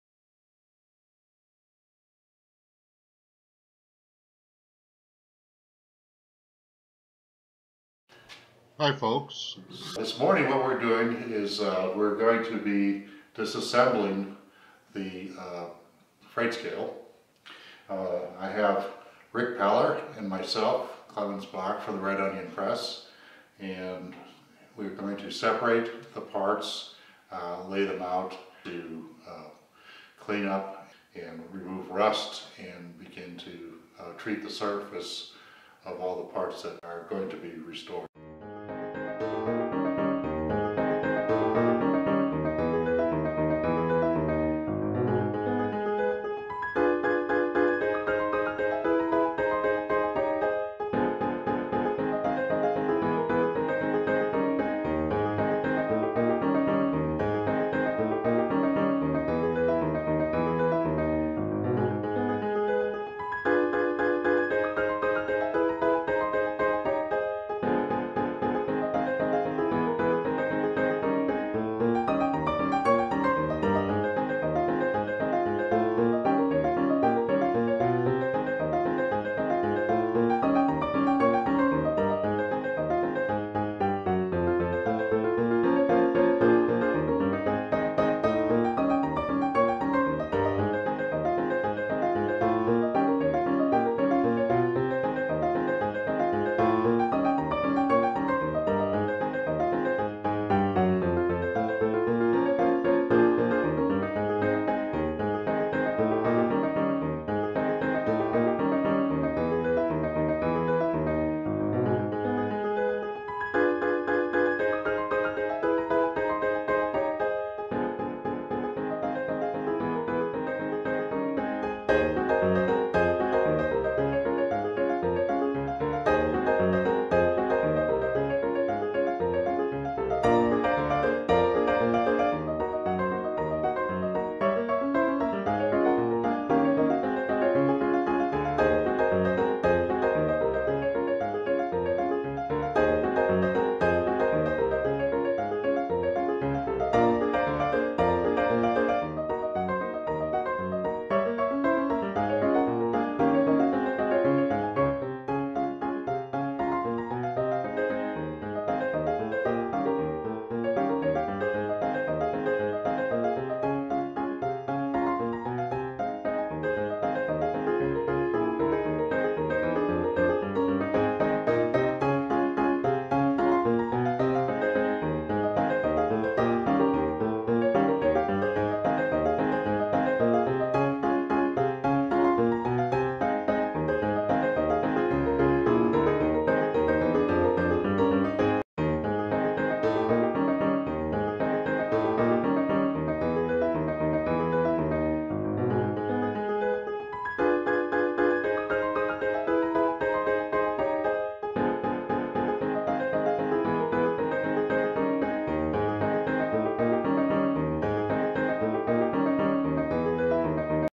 Restoration of the Acworth Depot Scale ~ Video 2
Disassembling the Scale to clean and restore the part.